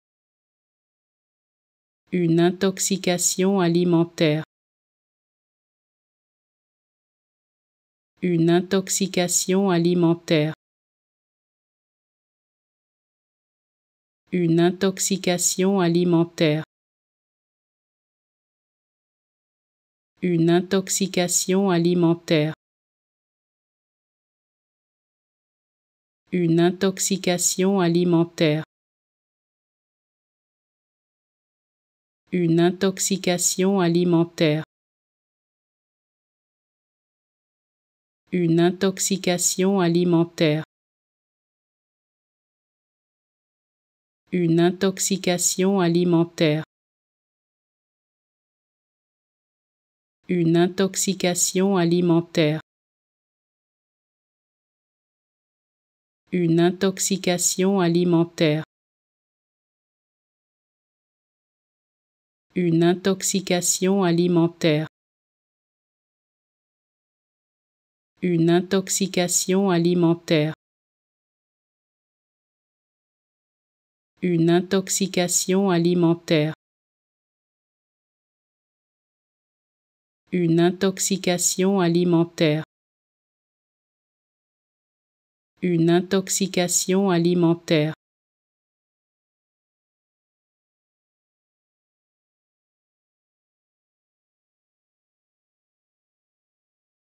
How to Pronounce ''Une intoxication alimentaire'' (Food poisoning) Correctly in French
Learn how to say and properly pronounce ''Une intoxication alimentaire'' in French with this free pronunciation tutorial.